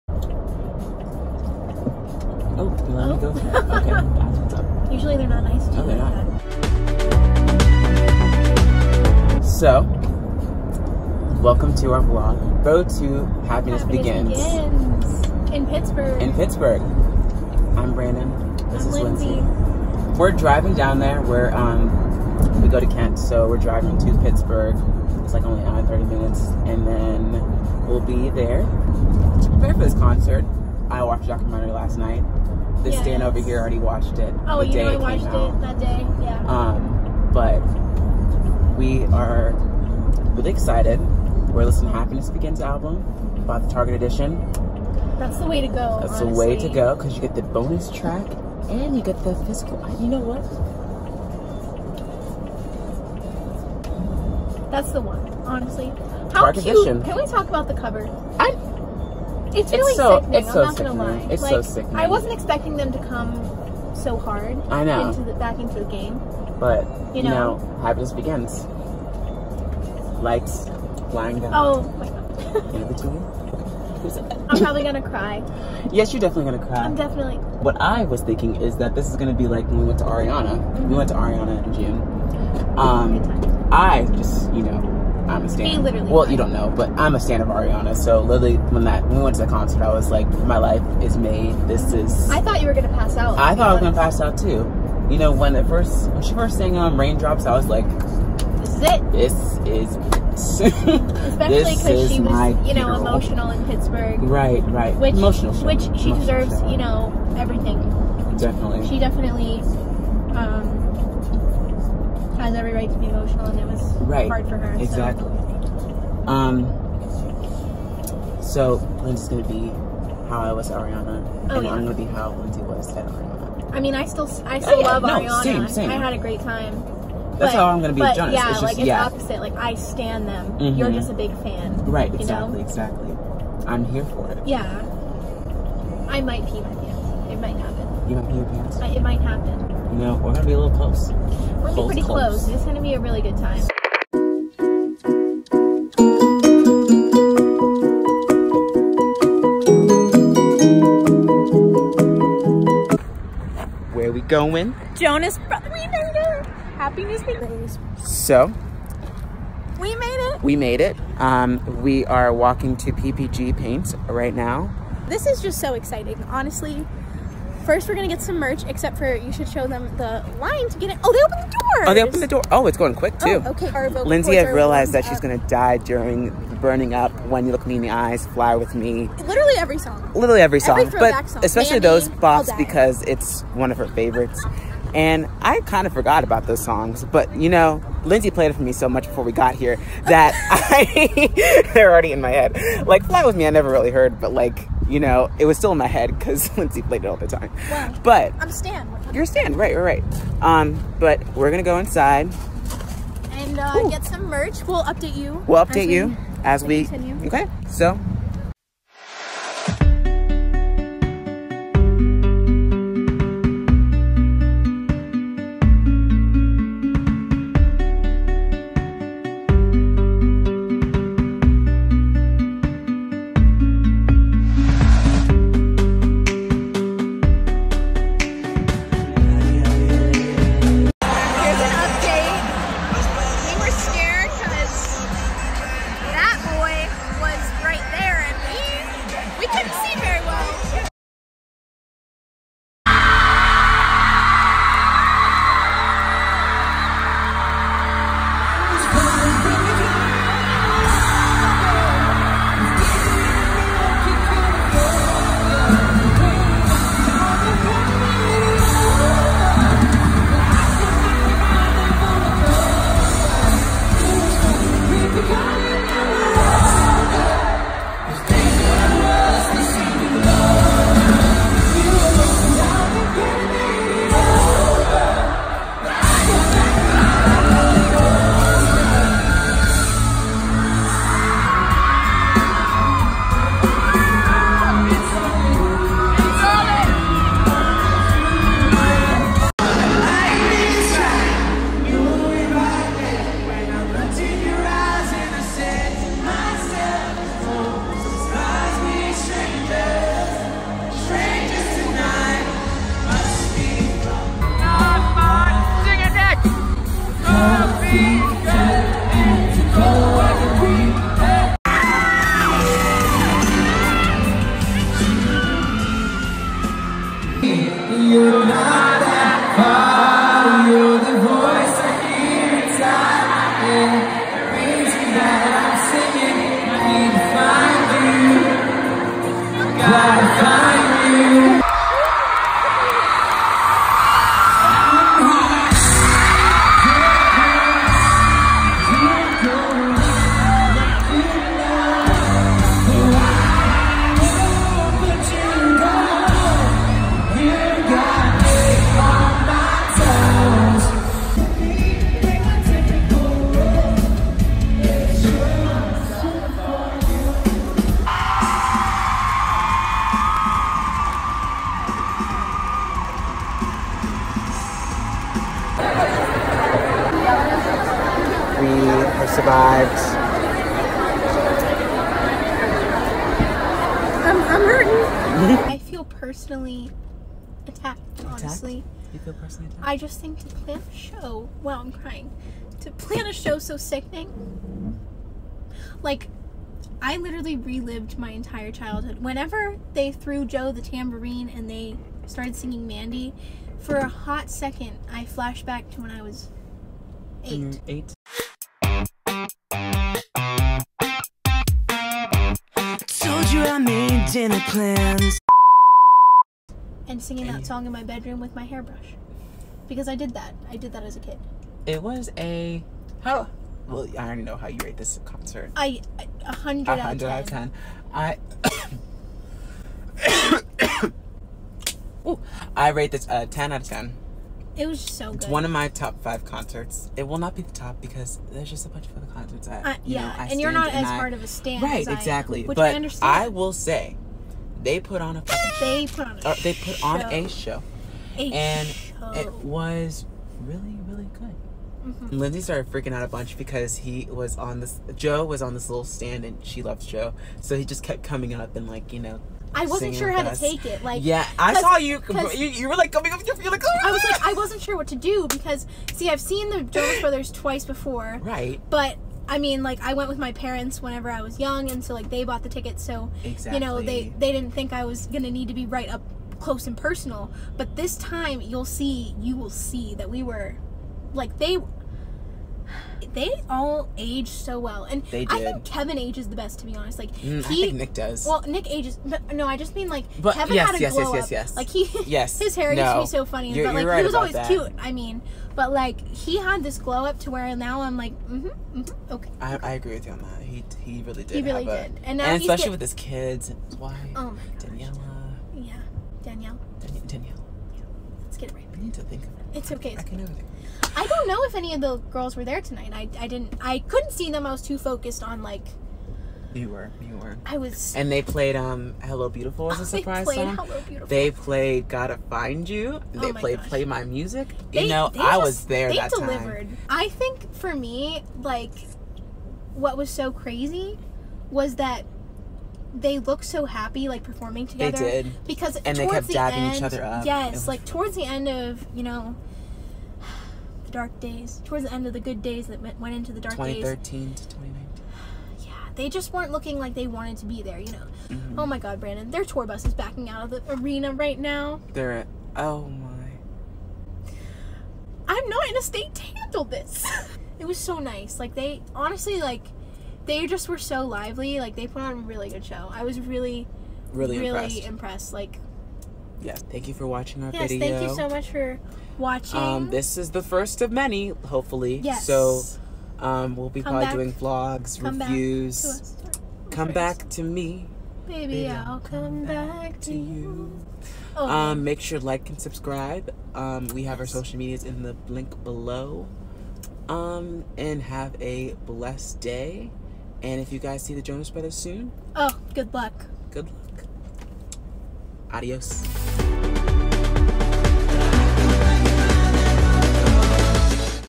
Road to Happiness Begins: A Concert Diary
Branden and Lindsey take on the Jonas Brothers' Happiness Begins Tour in Pittsburgh!!!
Branden: Instagram @brandentownes Twitter @branden_townes
Lindsey: Instagram @lindseysphotos_ Twitter @lindseyav44
Combined: Instagram @BnLReviews